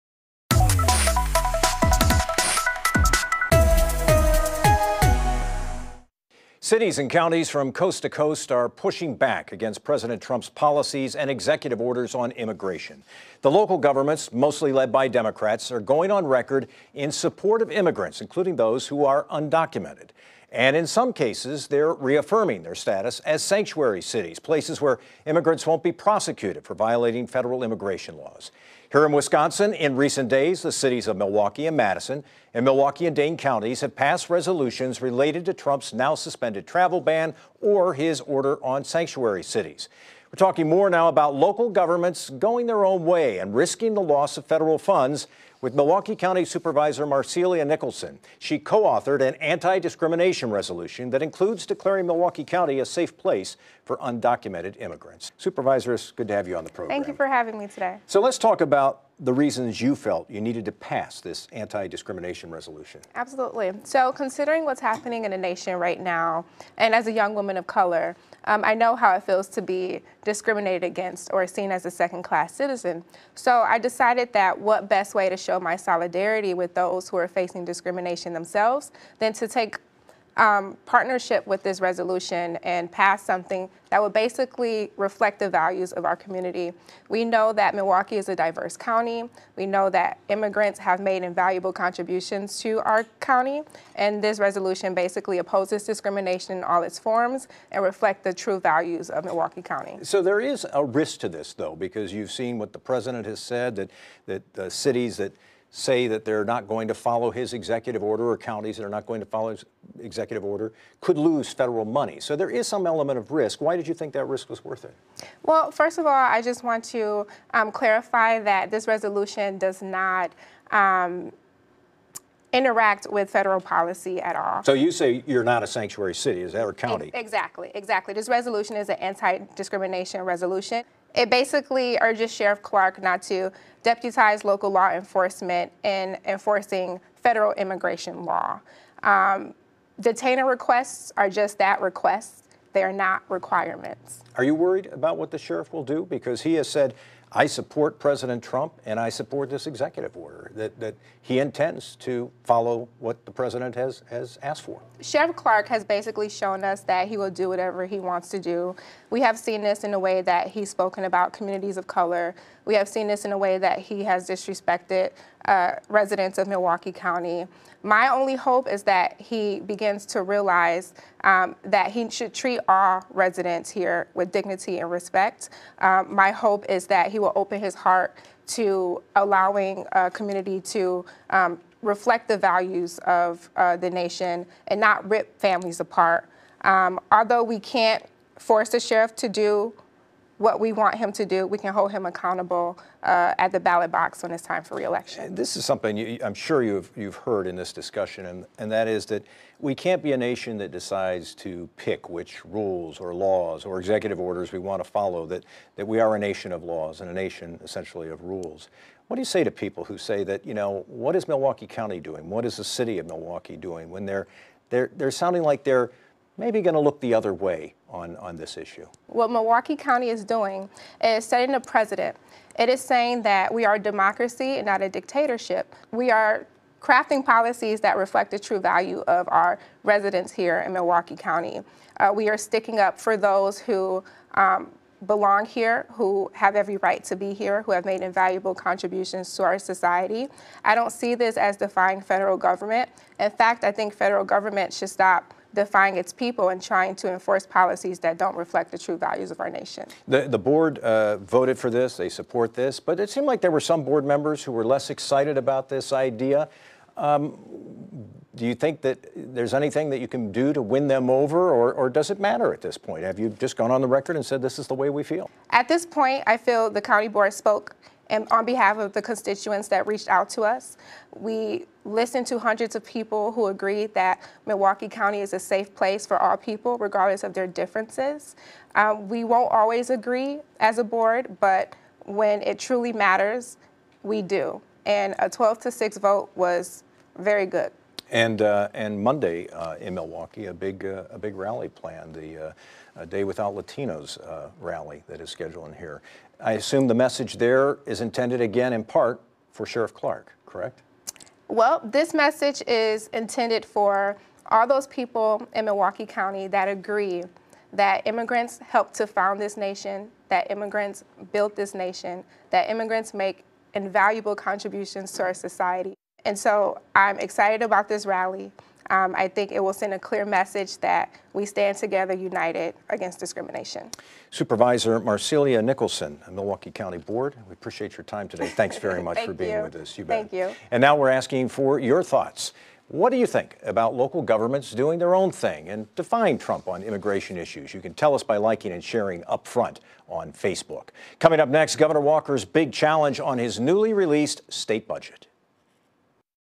Local board stands up for immigrants
Milwaukee Co. Supervisor Marcelia Nicholson says the county board passed an anti-discrimination resolution in response to President Trump's travel ban.